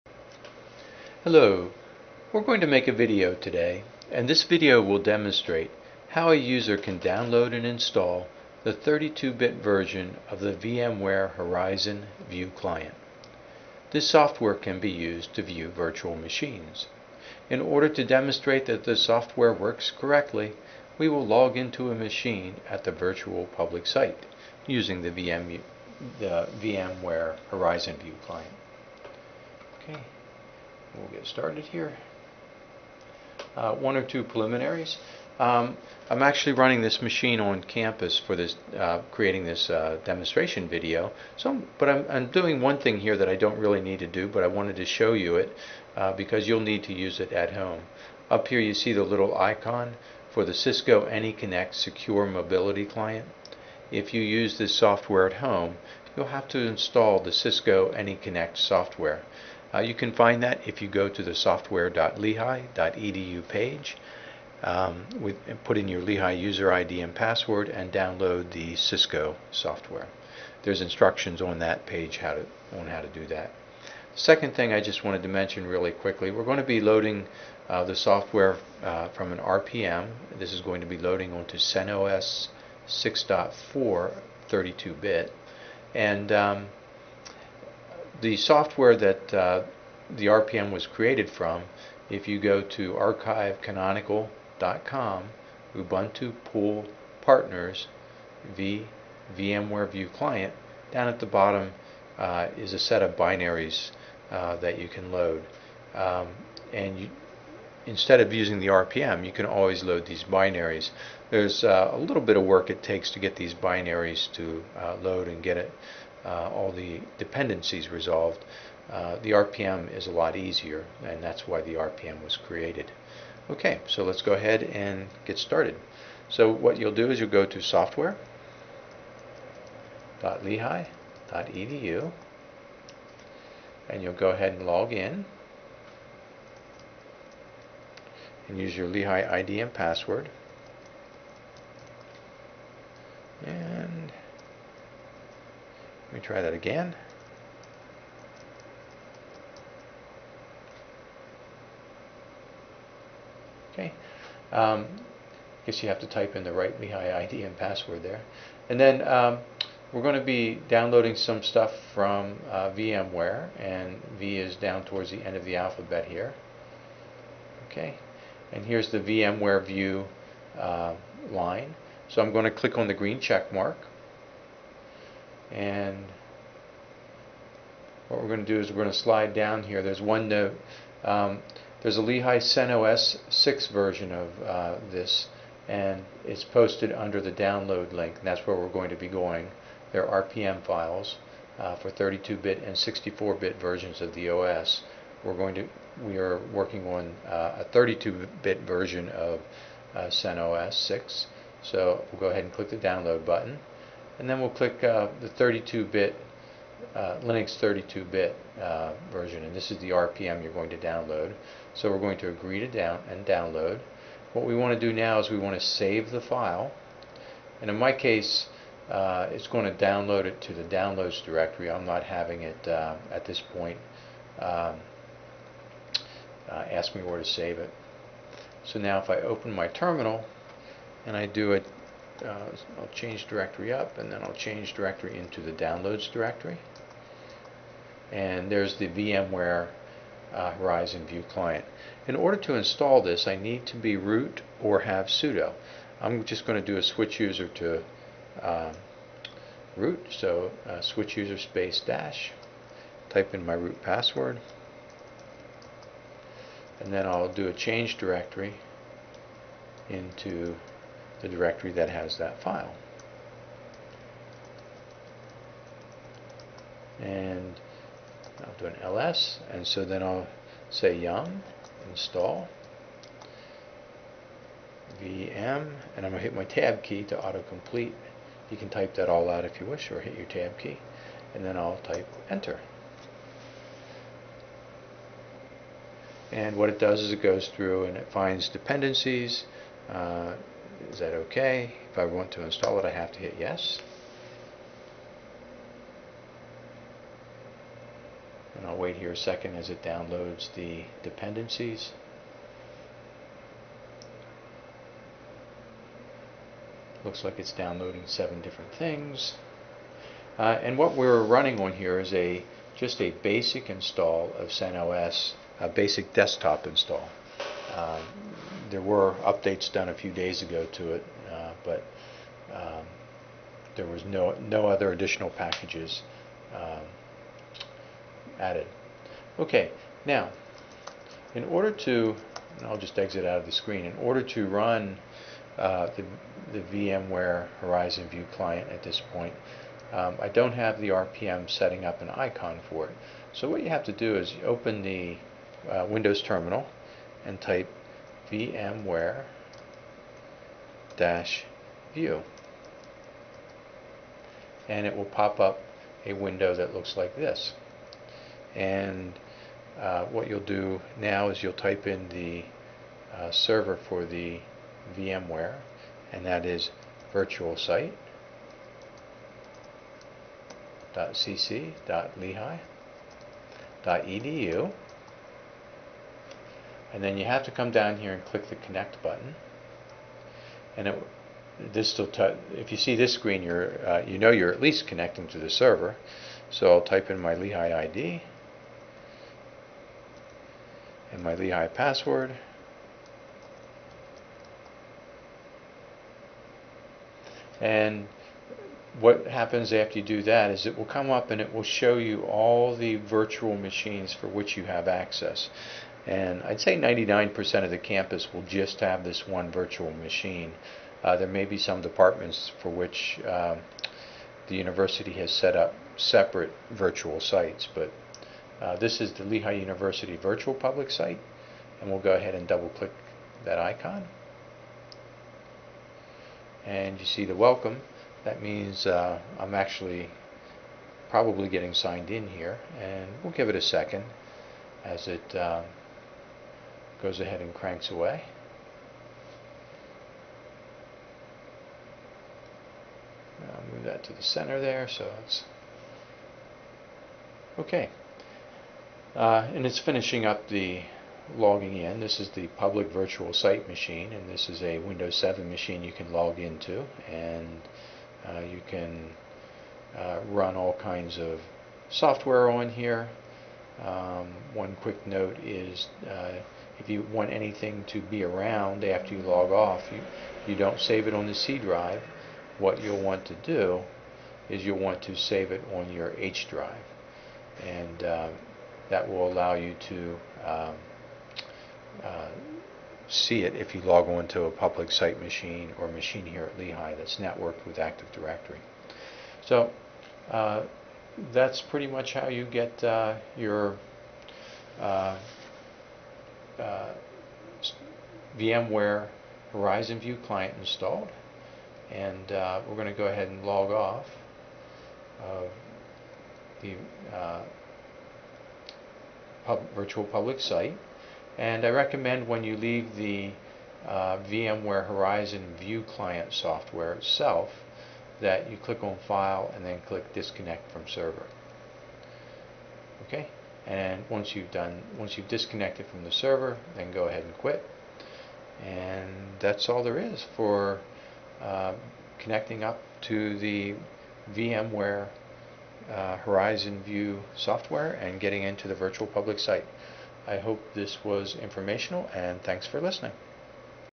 CentOS 6/Red Hat 6 32-bit RPM for VMware Horizon View Client 2.1.0
This video shows how to download and install a CentOS 6/RedHat 6 64-bit rpm for the VMware Horizon View Client 2.1.0 package.